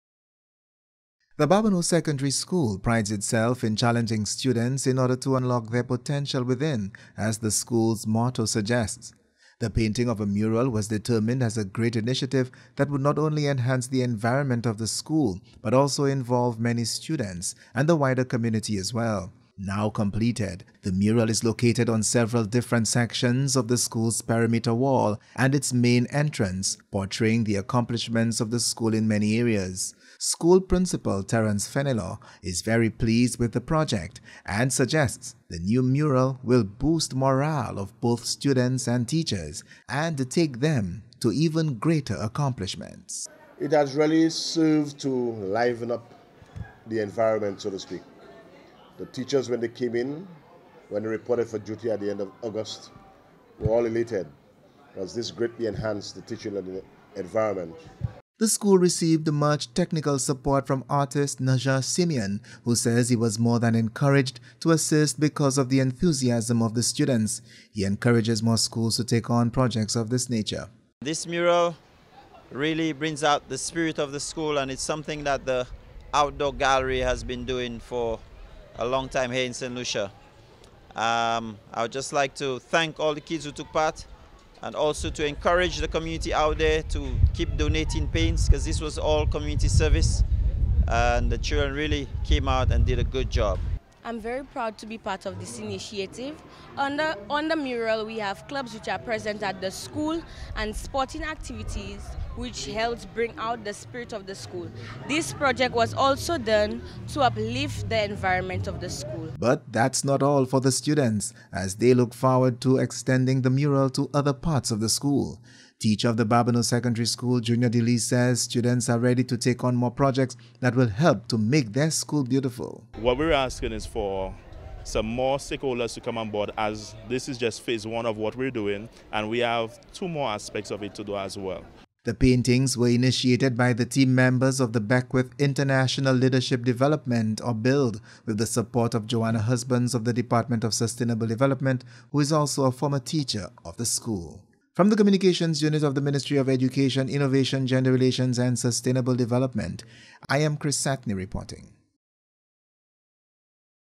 Babonneau Secondary Mural (St. Lucia)
The Babonneau Secondary school is celebrating the accomplishment of a major initiative that will help to transform the school’s appearance.

The painting of a mural was determined as a great initiative that would not only enhance the environment of the school but also involve many students and the wider community as well.

Now completed, the mural is located on several different sections of the school’s perimeter wall and its main entrance portraying the accomplishments of the school in many